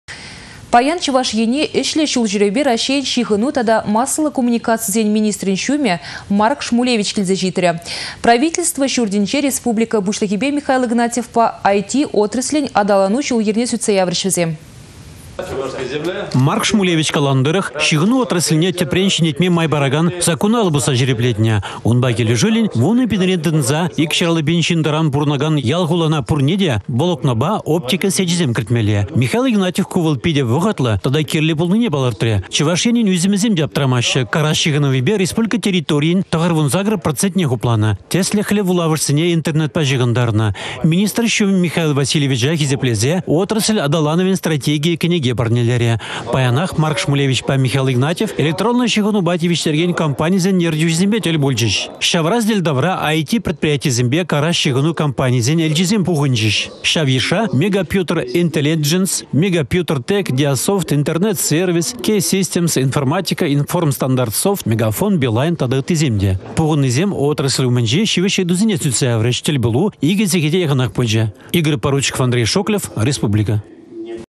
Шупашкарта - Шмулевич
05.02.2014
Паян Чăваш Ене ĕçлĕ çул çÿревпе Раççейĕн çыхăну тата массăллă коммуникацисен министрĕн çумĕ Марк Шмулевич килсе çитрĕ. Правительство çуртĕнче Республика Пуçлăхĕпе Михаил Игнатьевпа Ай-Ти отраслĕн аталану çул-йĕрне сÿтсе яврĕç